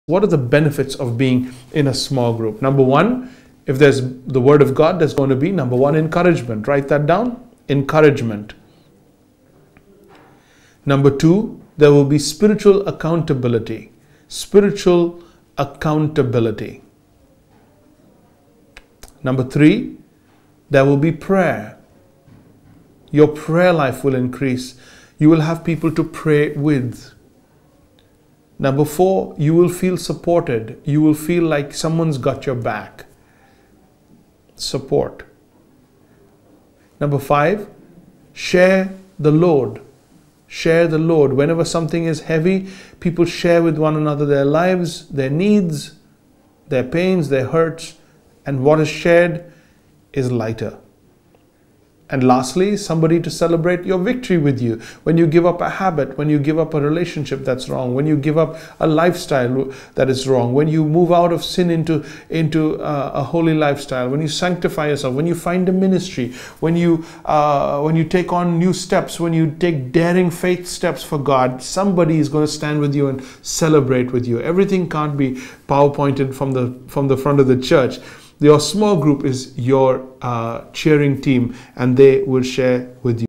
Benefits of being in a Small Group
1. ENCOURAGEMENT
2. SPIRITUAL ACCOUNTABILITY
3. PRAYER
4. SUPPORT
5. SHARE THE LOAD
6. CELEBRATE VICTORY